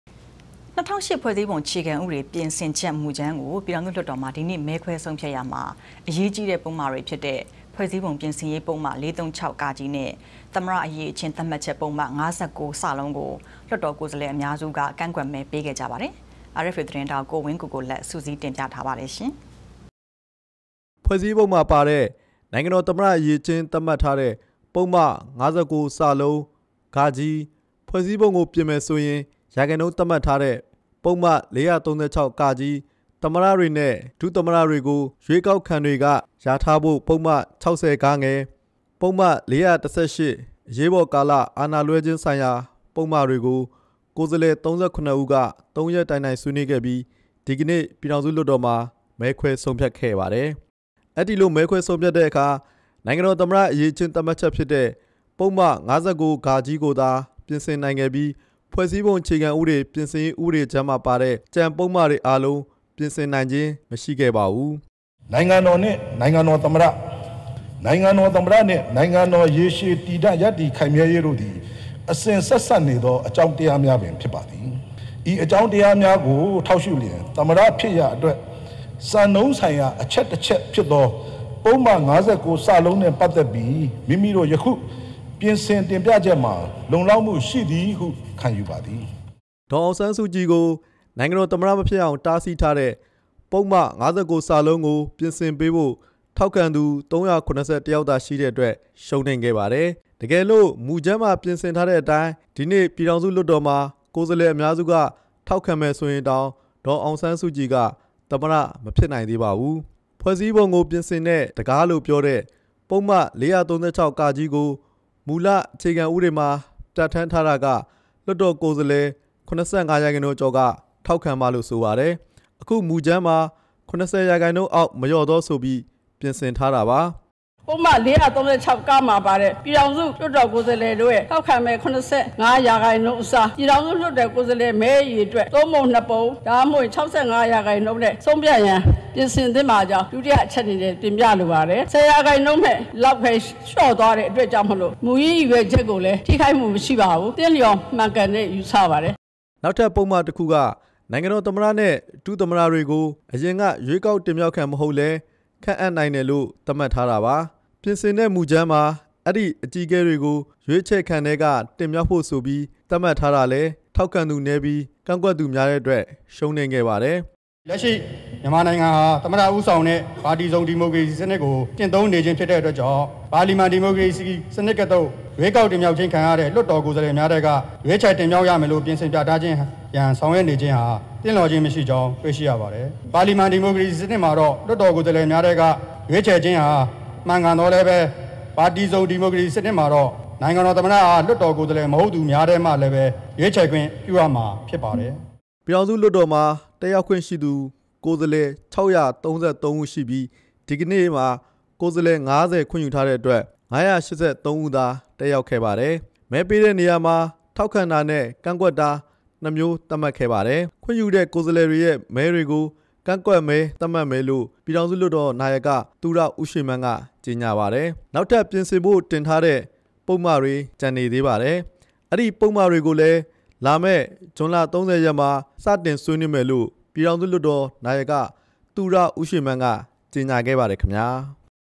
Parliament Rejects Five Constitutional Amendments
၂ဝဝ၈ ဖြဲ႔စည္းပုံအေျခခံဥပေဒ ျပင္ဆင္ခ်က္မူၾကမ္း ကုိ ျပည္ေထာင္စု လႊတ္ေတာ္မွာ ဒီေန႔ မဲခြဲဆံုးျဖတ္ ရာမွာ အေရးႀကီးတဲ့ပုဒ္မေတြျဖစ္တဲ့… ဖဲြ႔စည္းပံုျပင္ဆင္ေရး ပုဒ္မ ၄၃၆ (က) နဲ႔၊ သမၼတအရည္အခ်င္း သတ္မွတ္ခ်က္ ပုဒ္မ ၅၉ (စ) ကို လႊတ္ေတာ္ကုိယ္စားလွယ္အမ်ားစုက ကန္႔ကြက္မဲေပး ခဲ့ၾကပါတယ္။ RFA သတင္းေထာက္ ကုိဝင္းကုိကုိလတ္ စုစည္းတင္ျပ ထားပါတယ္။